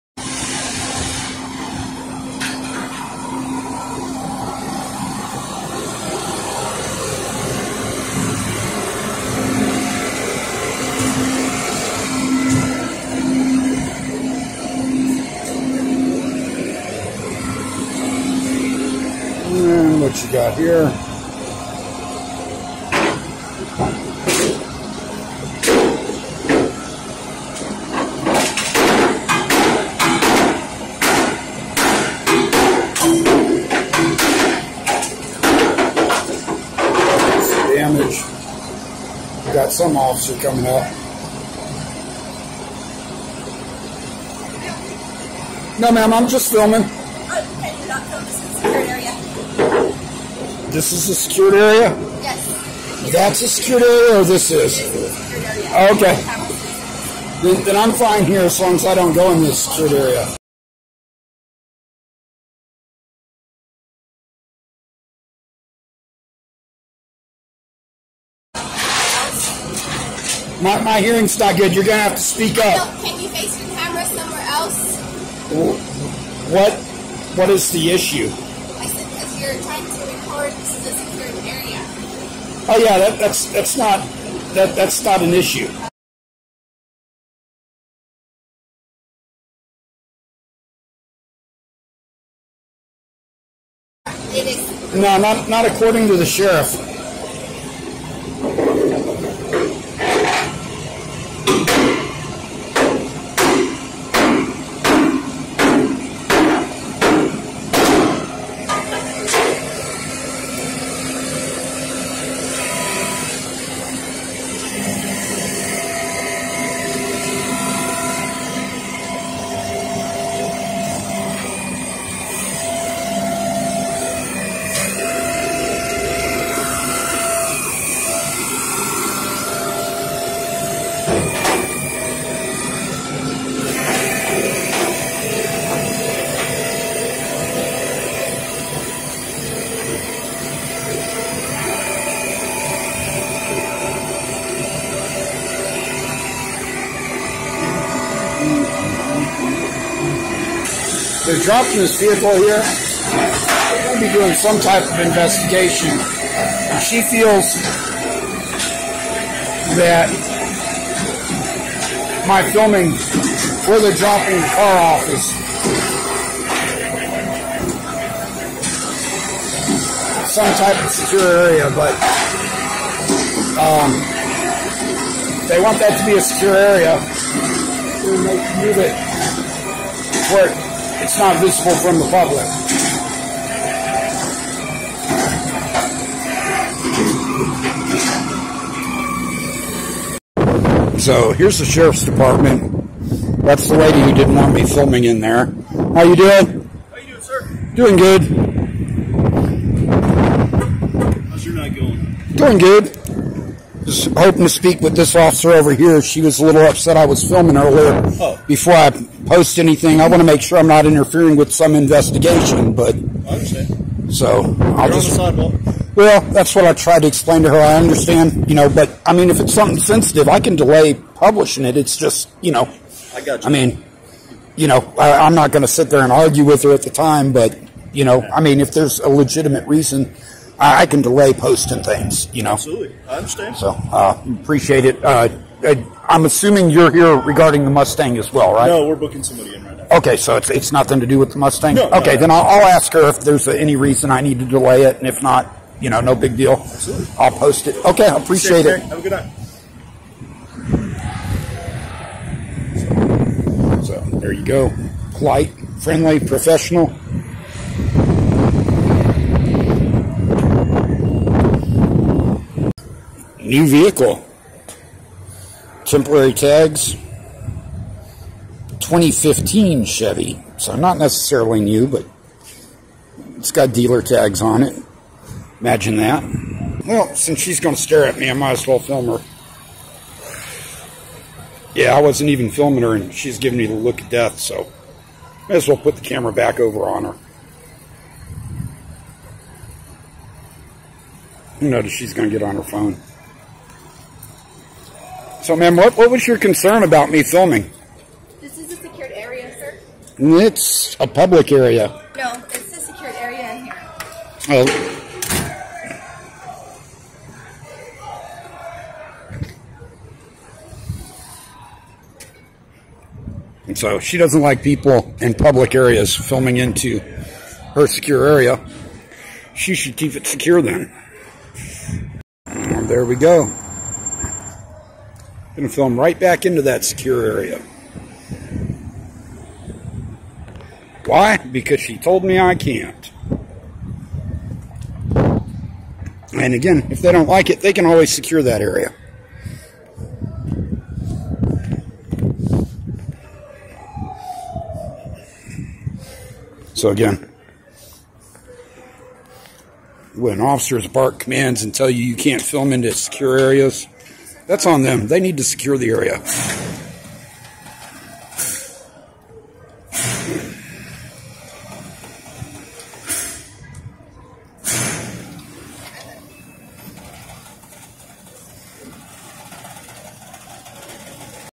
Bexar county super secret car jail. No filming, breathing, walking or looking allowed.
This is where they take your car to pull information stored on your vehicle, like speed and gps locations.
  Of course they feel us citizens have no right to
see this location.   The female deputy tried to stop my filming, of course that just made me film more.
  If she could have given me a reason not to film this super secret classified restricted area I may have just deleted the footage.
  Facts are it just shows some equipment, nothing super sensitive in there, if there was sensitive equipment then they need to move it to where it can not be seen from the public.
  The other undercover officer was polite professional and acknowledged my right to record from a public area, to me that is a pass.
  What is a fail is an officer attempting to intimidate under the guise that it is wrong to film in public because you can see into a restricted area.
  I can film planes by the airport.  I can not walk onto the runway.   I can film the jail, I can not walk inside the jail.  Her failing to understand that is a concern.
  In her mind officers can tell someone anything and if they fail to comply it is a crime.